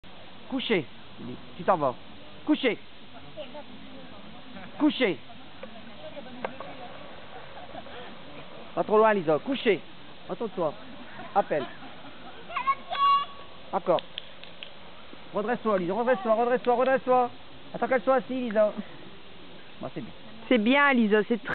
lisa.MOV
lisa (bientot 4 ans) fait un rappel avec ficelle (9 mois ...) sa petite Jack russell